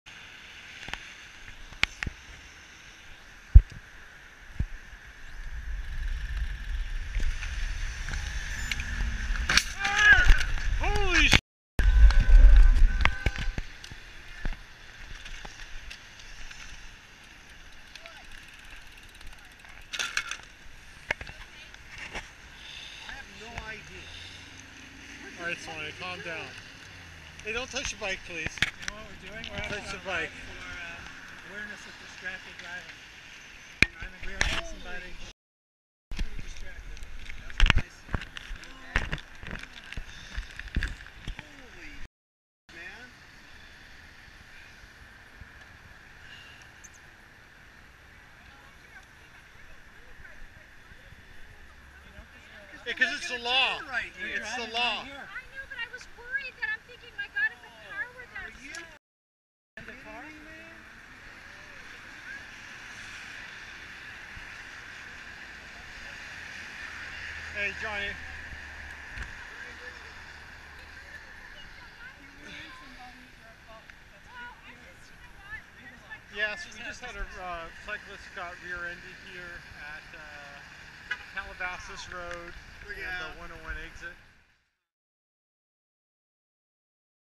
Cyclist hit by Distracted Driver during Distracted Driving Ride
In the very definition of irony, a cyclist was hit by a distracted driver at the end of the ride to benefit the Milt Olin Foundation, dedicated to fight distracted driving.